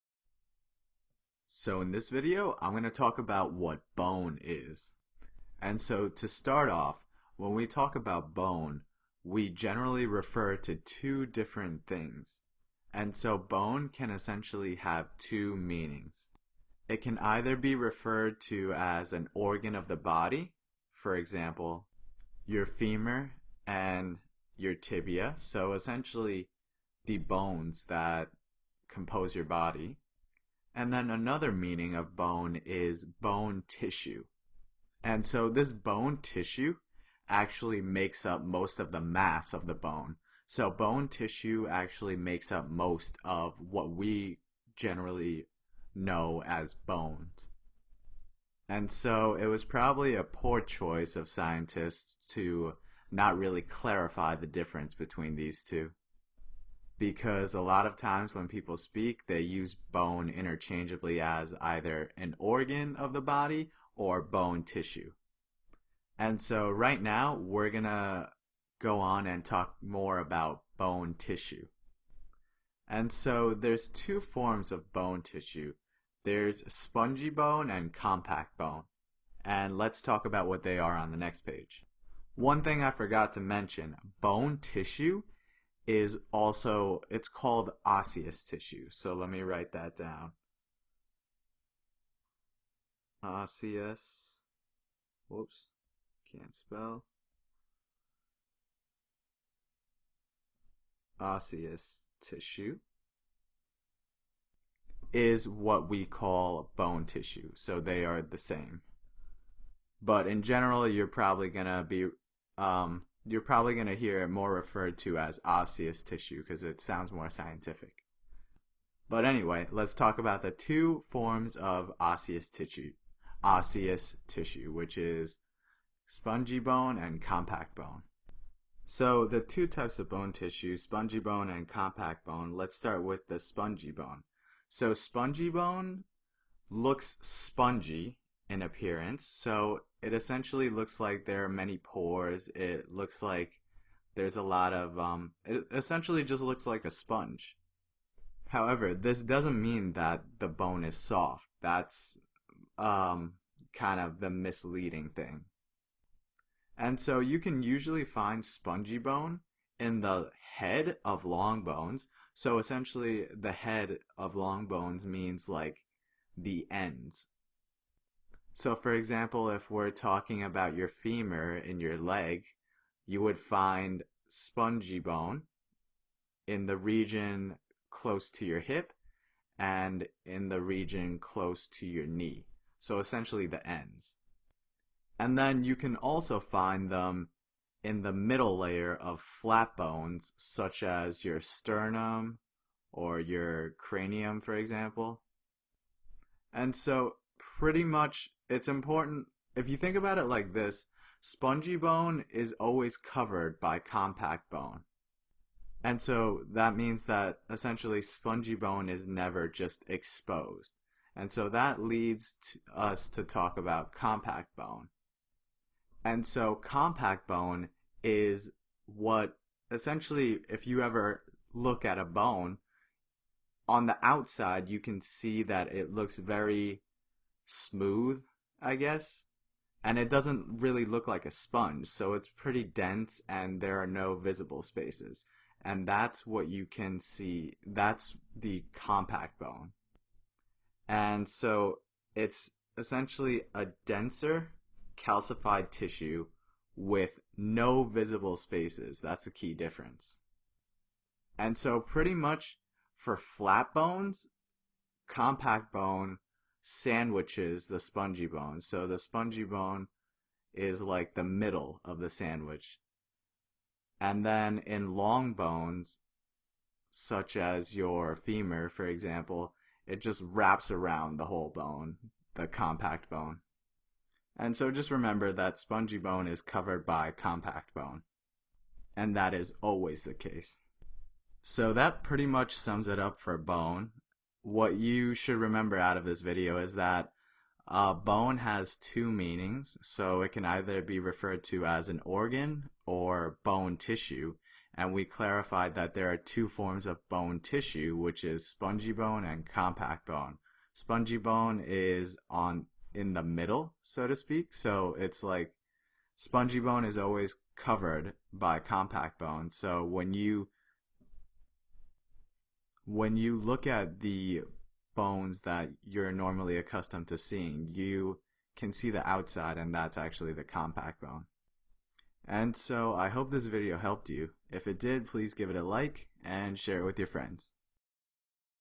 Bone Tissue
This video explains what bone tissue is and explains what spongy bone and compact bone is